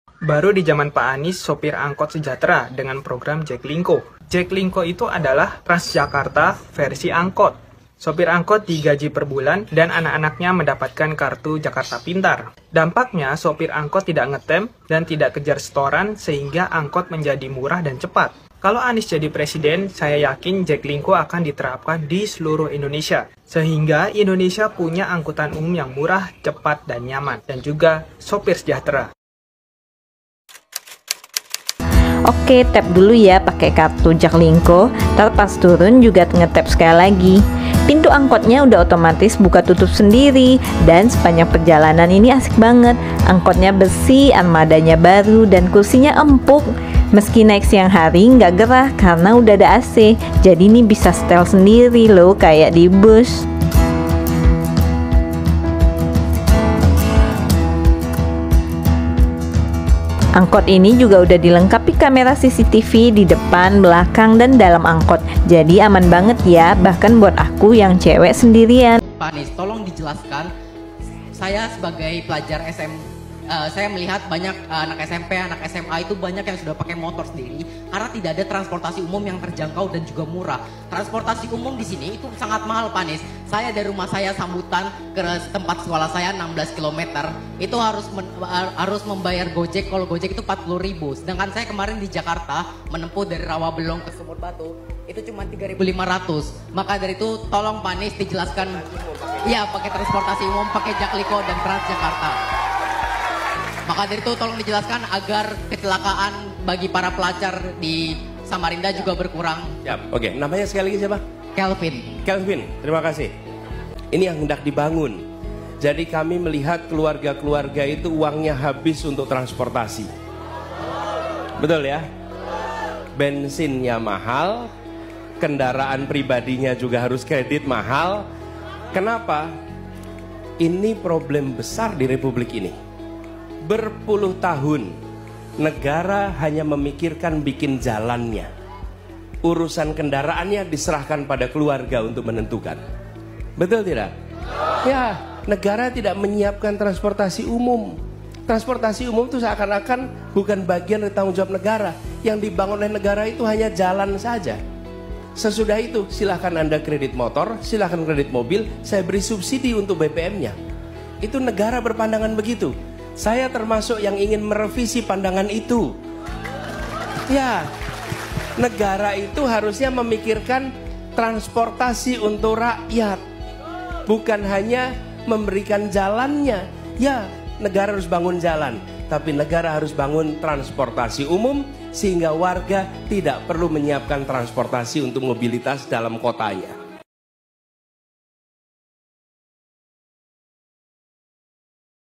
Pendukung Prabowo Kaget, Anies Gaji SUPIR ANgkot di Jakarta
Di jakarta itu ada angkot yg namanya Jaklingko,angkotnya ber AC,keliling Jakarta bisa cukup 3.500 loh, gratis untuk rakyat kecil untuk karyawan,kebijakannya menguntungkan masyarakat bawah,dan supir angkotnya di gajih UMR Jakarta,yg begini masih di bilang tukang bohong,yg kerjanya cuma bisa ngomong katanya.
Anies baswedan tenang serius tuntas